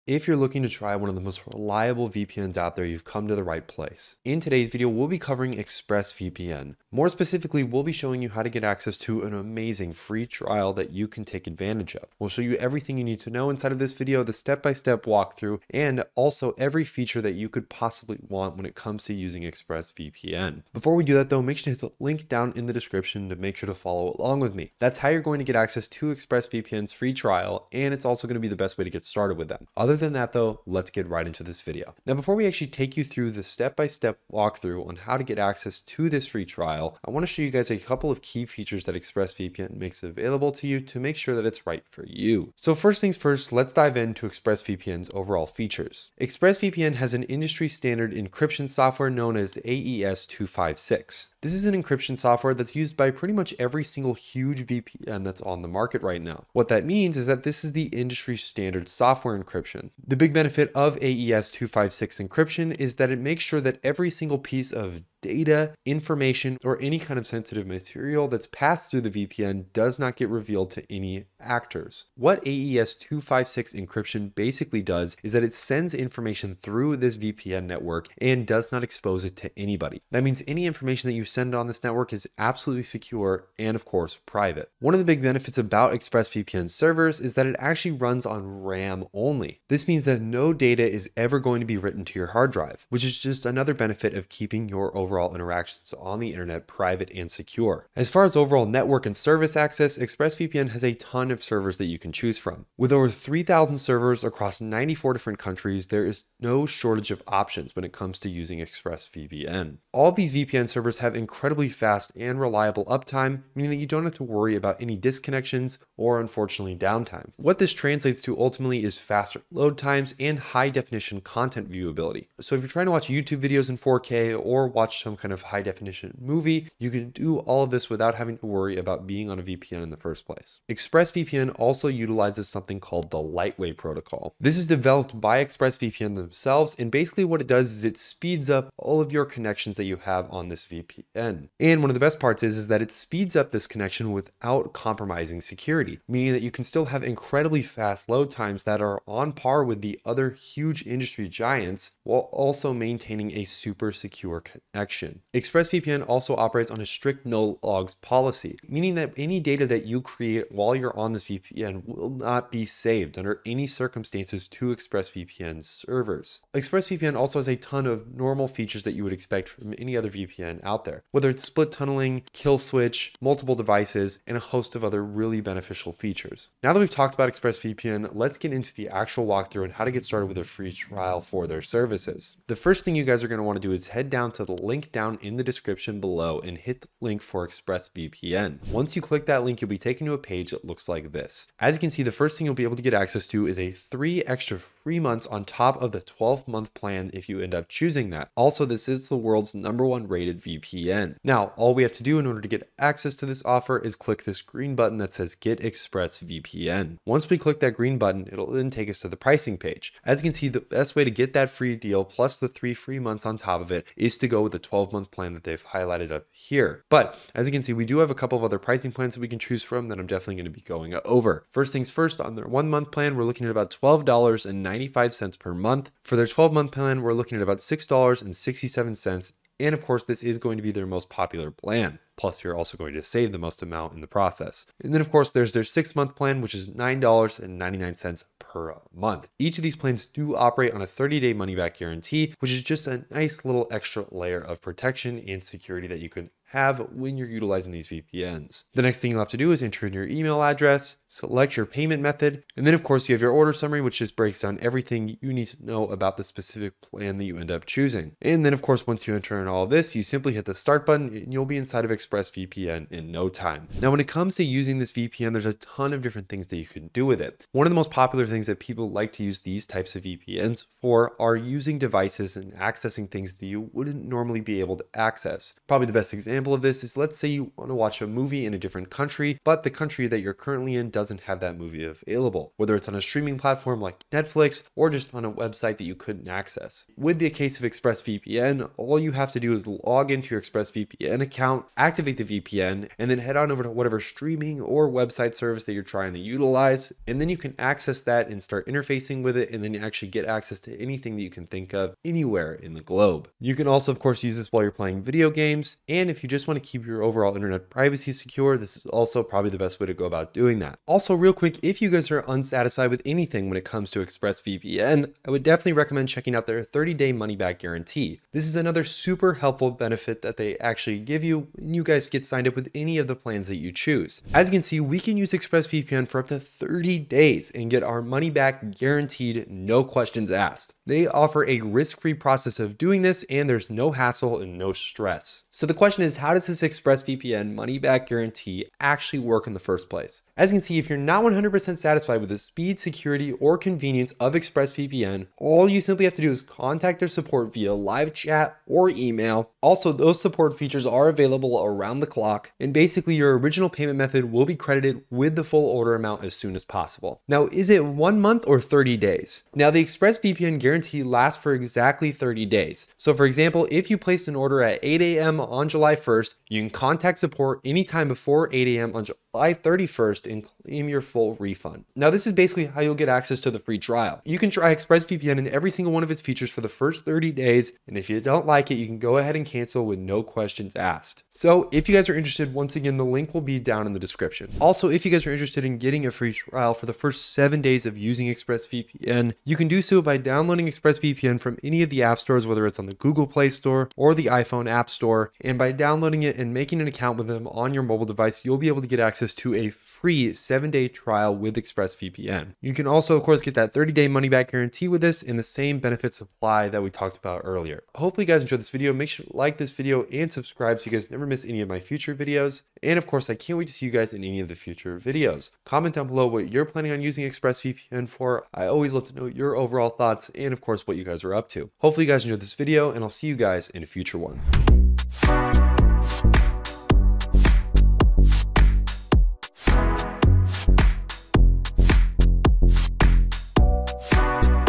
How to Get an ExpressVPN Free Trial Account in 2025 (Step by Step Tutorial)
In this video, I go over a step-by-step tutorial on how to get an ExpressVPN free trial account in 2025. I'll guide you through the entire process, from signing up to exploring its key features.

Our Top VPN Recommendations:
🔵 NordVPN (Get up to 4 months FREE on yearly plans):

This tutorial is essential if you're looking to try out a top-tier VPN service without any financial commitment. ExpressVPN offers fast speeds, robust security, and the ability to unblock content globally, all of which you can experience during the free trial. Be sure to watch this video all the way as I cover how to cancel the trial before it ends, ensuring you won't be charged.

Learn how to safeguard your online privacy and enhance your internet experience with ExpressVPN's free trial. By following this guide, you can unlock the full potential of a secure and private browsing experience.

Our channel is dedicated to educating you on everything there is to know about VPNs and ways to secure your online activities.

- Kyle

Timeline:
0:00 - Introduction
0:31 - Key Features
2:56 - How to Get Access to Free Trial (Walkthrough)
5:10 - 30 Day Money Back Guarantee
6:24 - 7 Days Free Tiral
6:47 - Conclusion